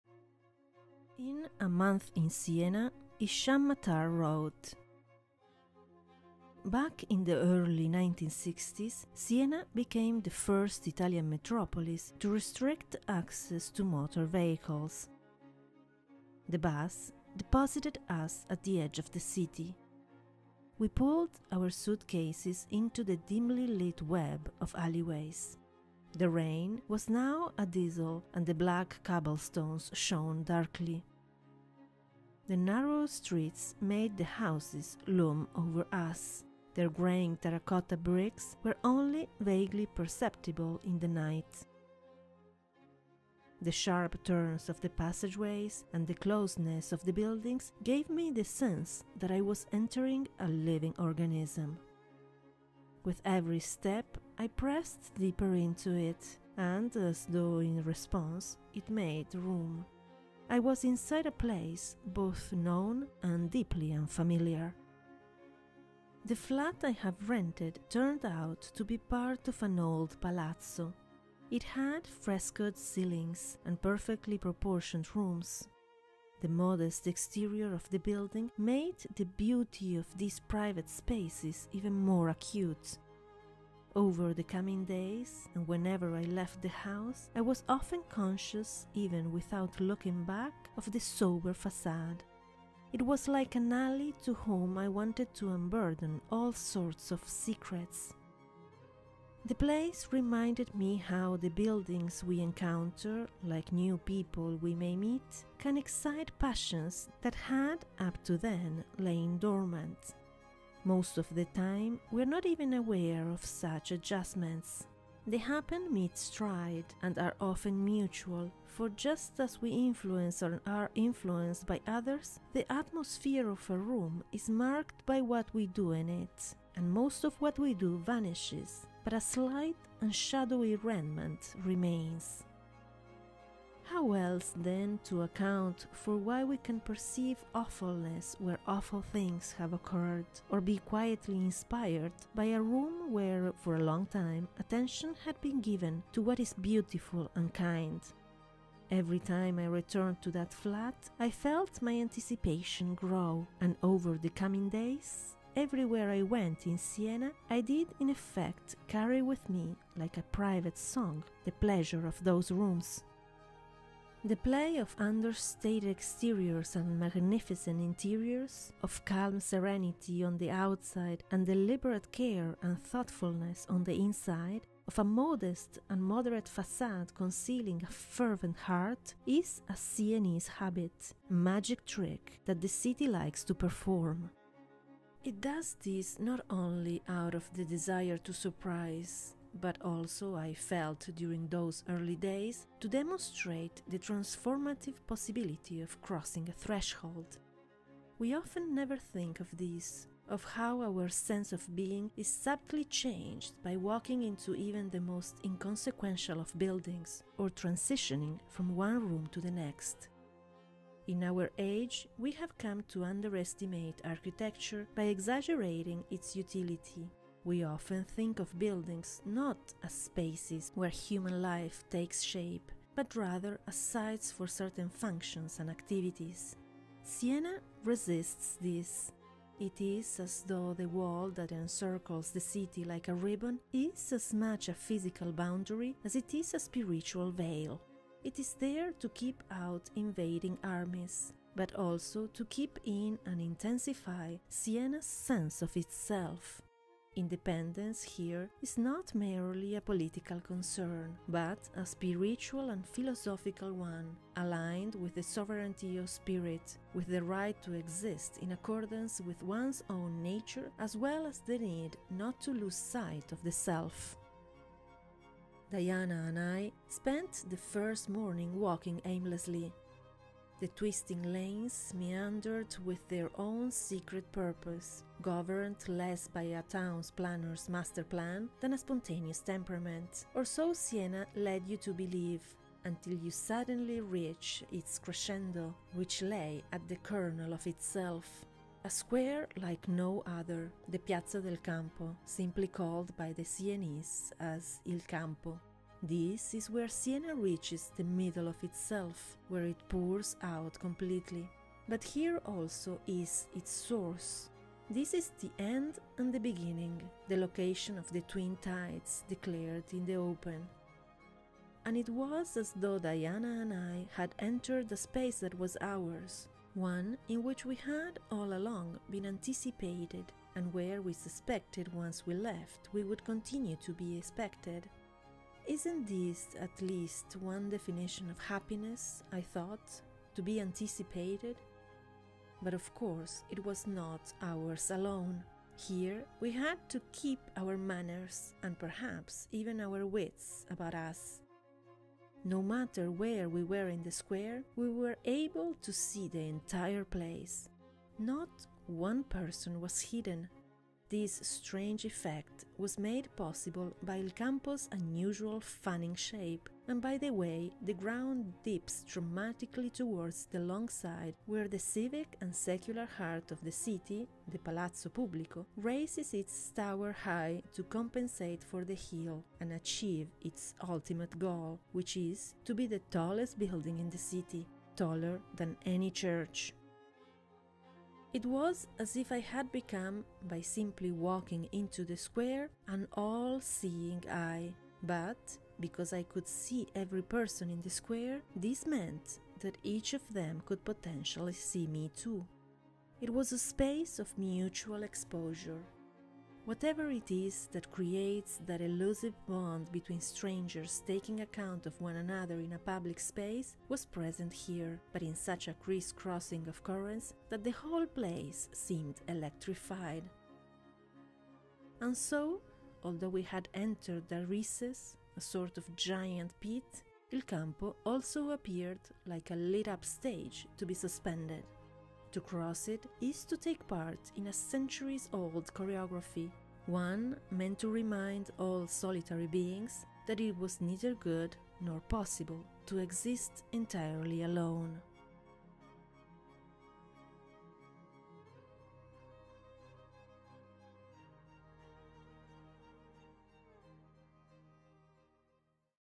A Month in Siena
In this video I read "The Shape of a Room" in A Month in Siena, by Hisham Matar published by Penguin Books. All images are taken from Villages and Towns, issue 4: Italy (Siena, San Gimignano, Assisi) by Yukio Futagawa, Published by GA